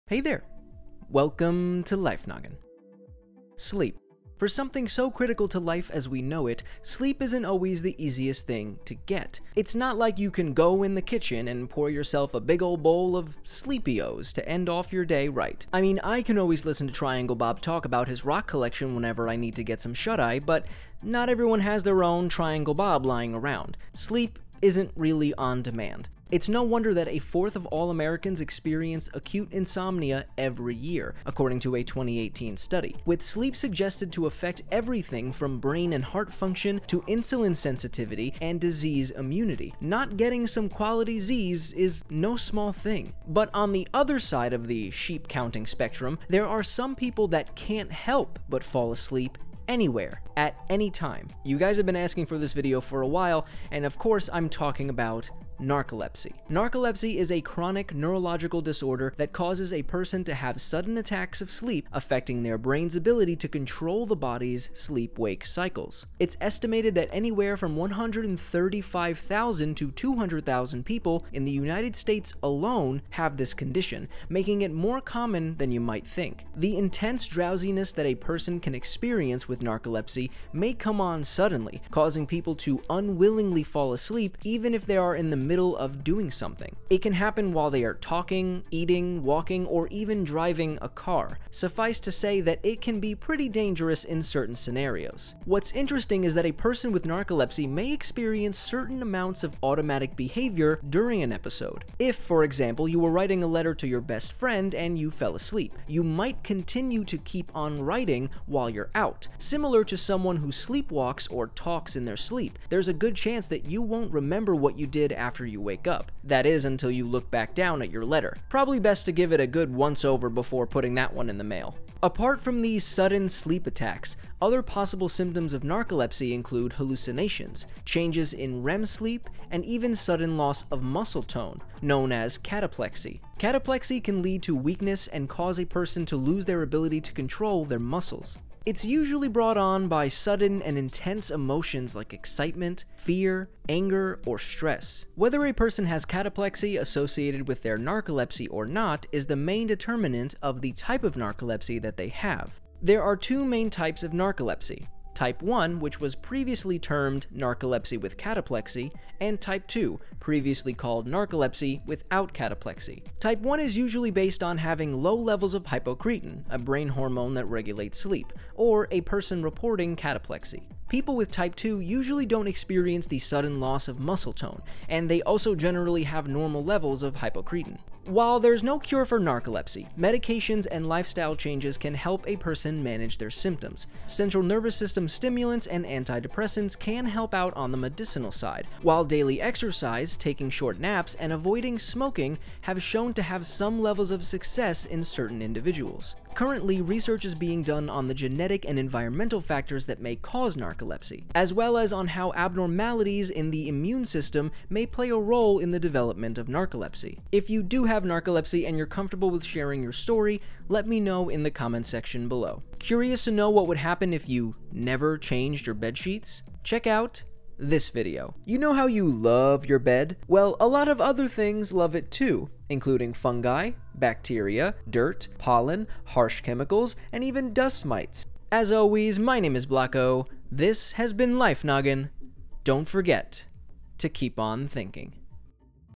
This Rare Disorder Makes You Fall Asleep Randomly
You guys have been asking for a video on Narcolepsy. Hopefully this shines some light on the topic!